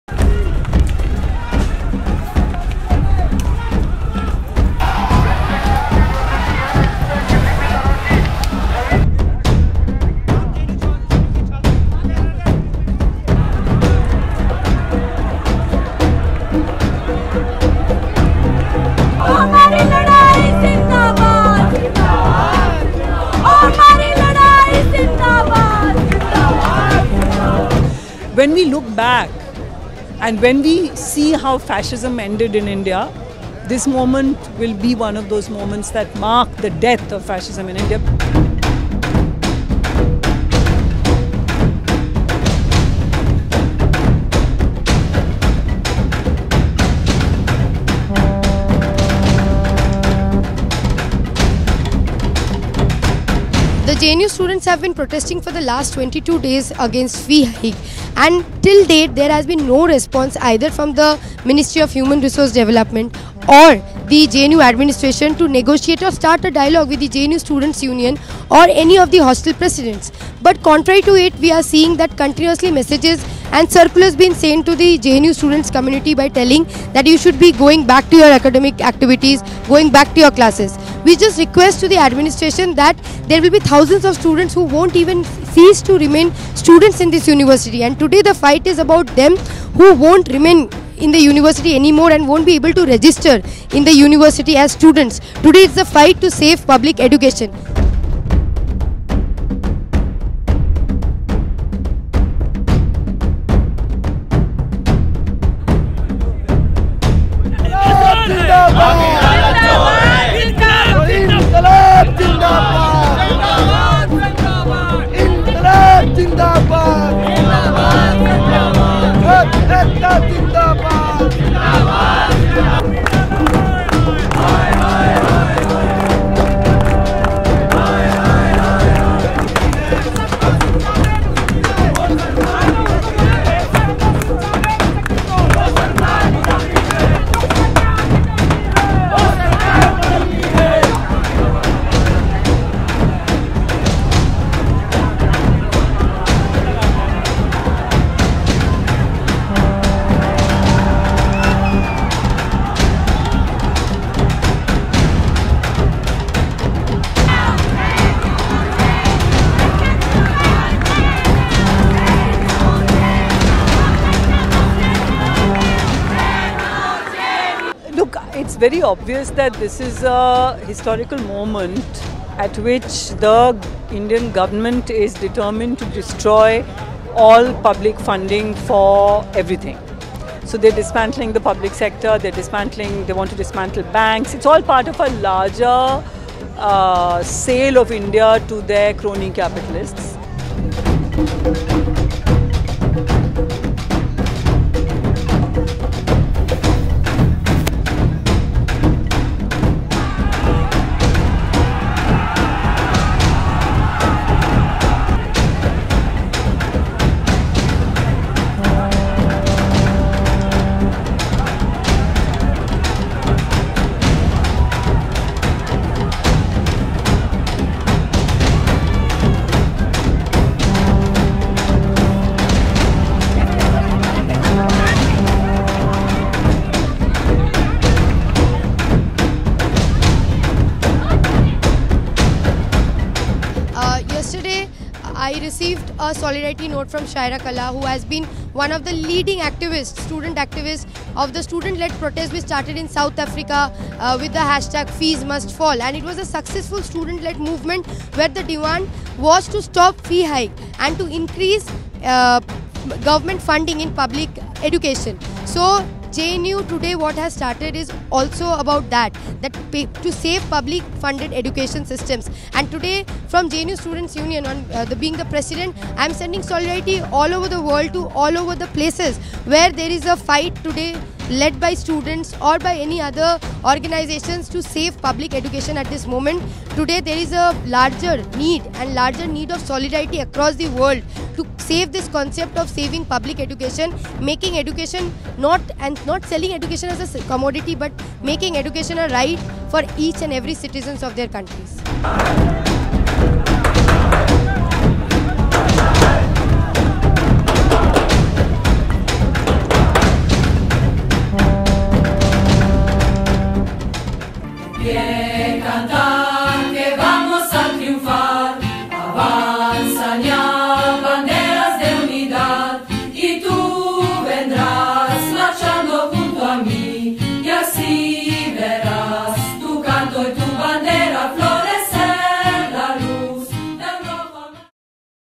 FeesMustFall: No barricades can stop JNU students' resistance
On Monday, November 18, more than 3,000 students and teachers of the New Delhi-based Jawaharlal Nehru University marched to the Indian parliament against the hostel fee hike and demanded free education for all. The march was brutally attacked by the Delhi police and the CRPF paramilitary force. Despite the massive repression, students continued their protest march until they reached within 2 km of the Indian parliament. More than 100 students were injured in the police action and around 10 students received critical wounds